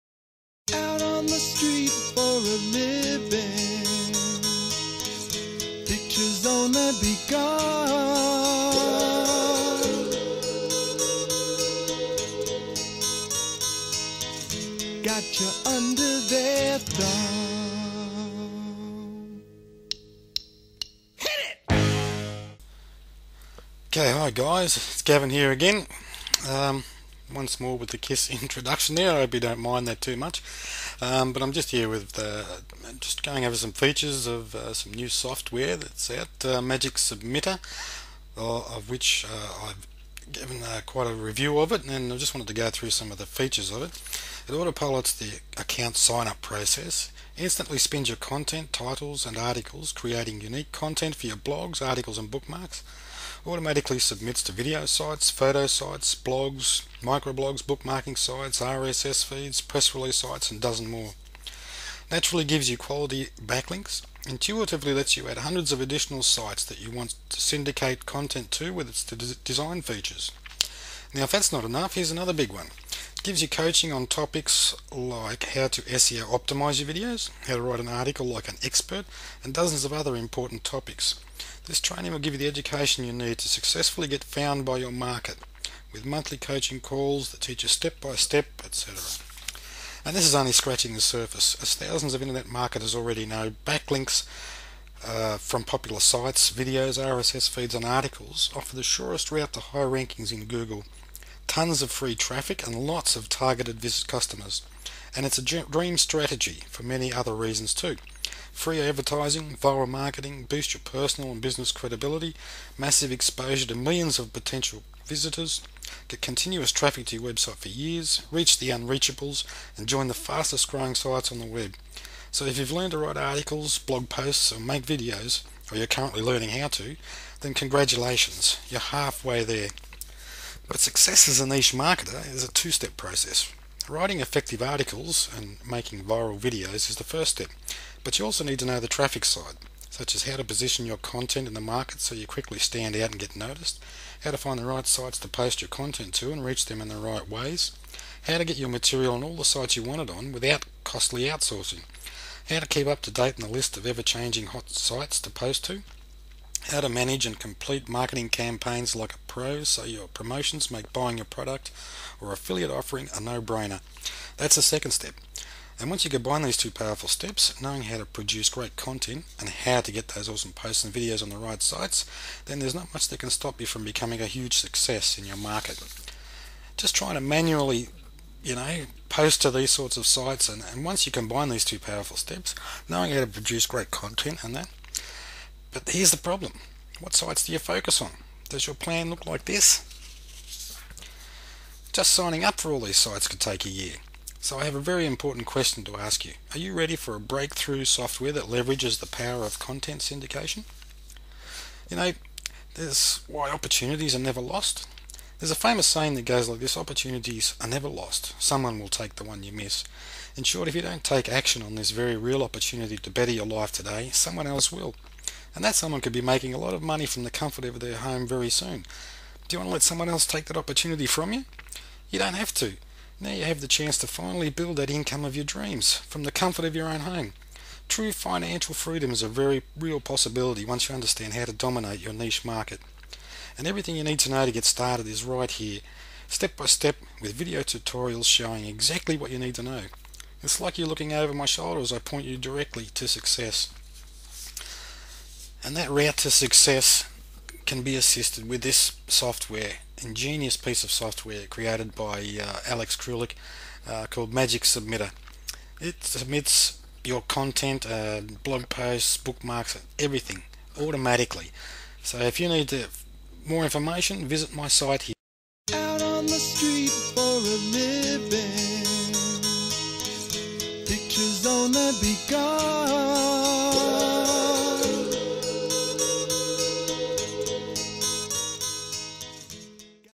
Magic submitter review
read my review of Magic Submitter before you consider buying. Magic submitter review by a long time user of the software, is it all it's cracked up to be?